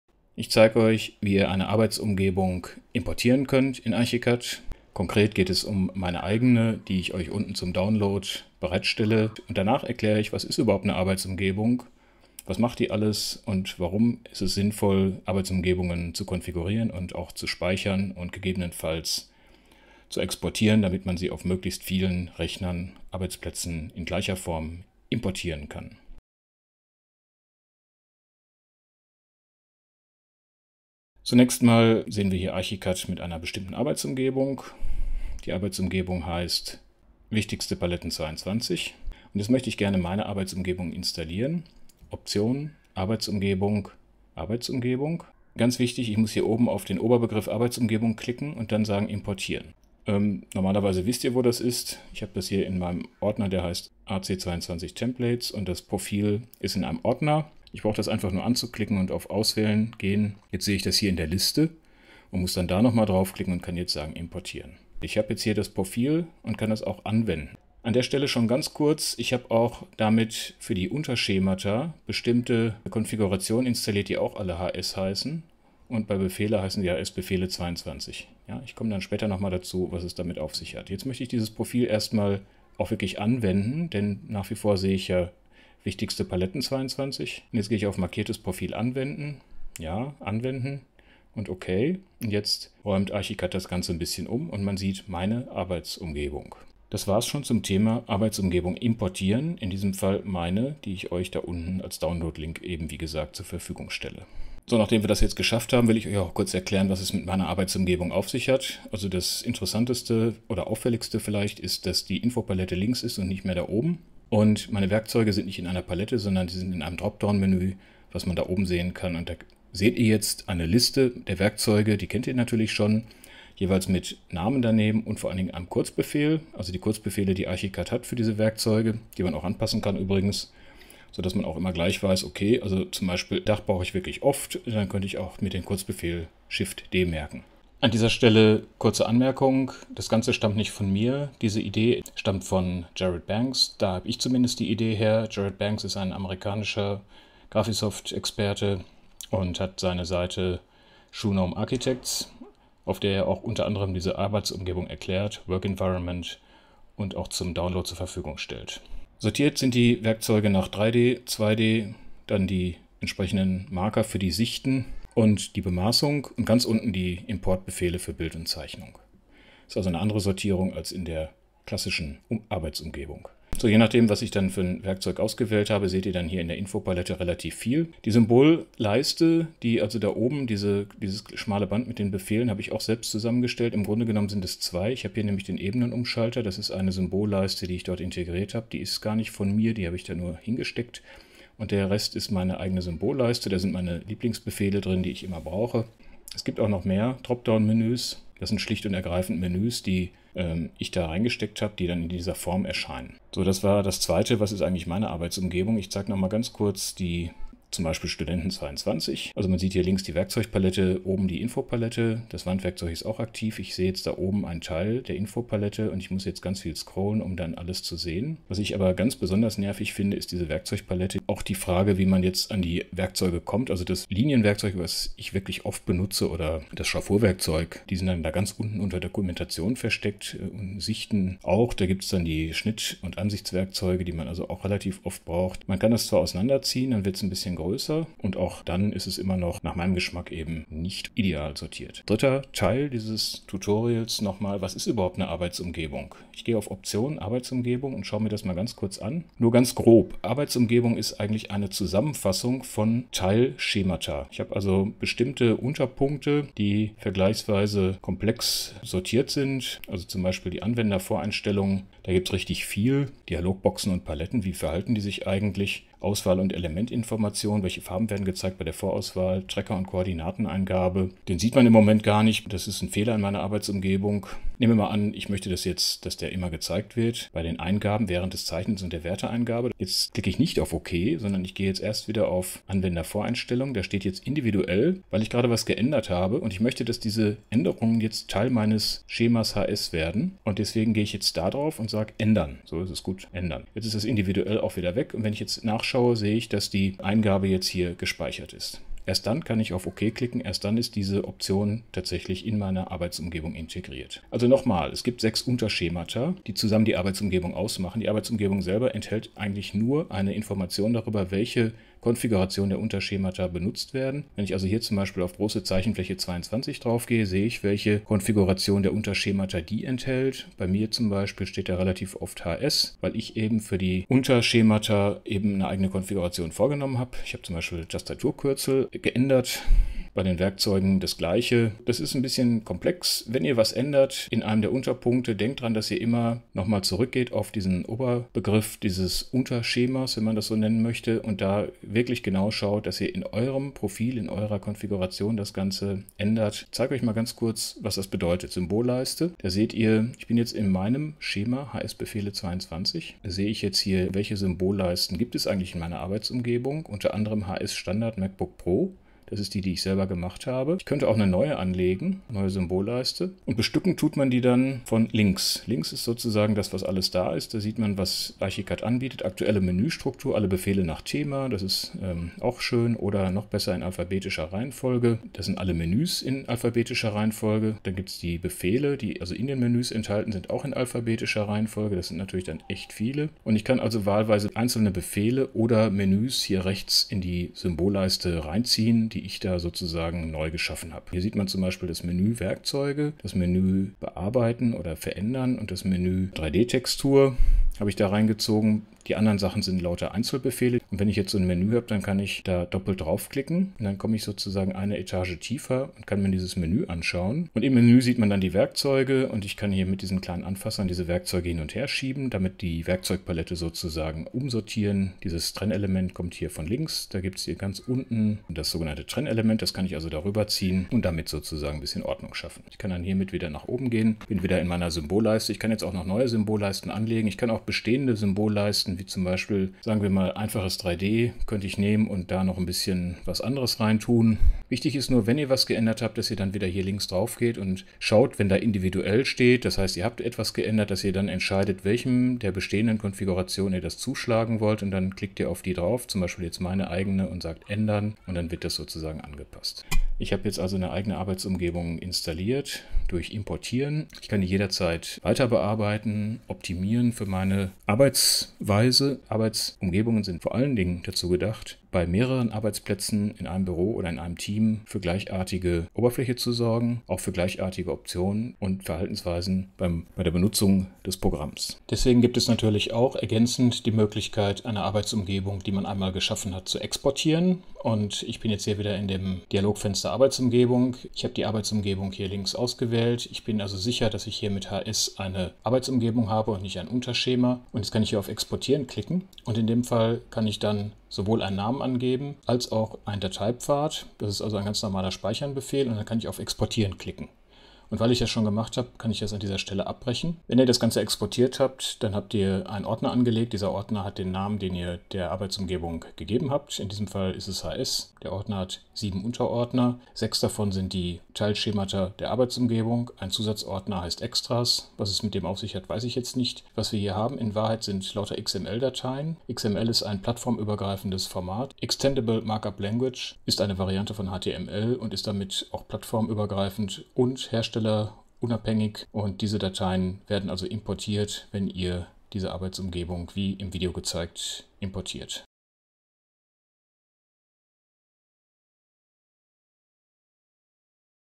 ARCHICAD BIM: Arbeitsumgebung
ARCHICAD's Arbeitsumgebung speichert fast alles, was es an Voreinstellungen gibt, auch Eure bevorzugte Programmoberfläche mit Paletten, Menüs und Symbolleisten. Im Video zeige ich Euch meine eigene Arbeitsumgebung und wie Ihr sie bei Euch installieren könnt.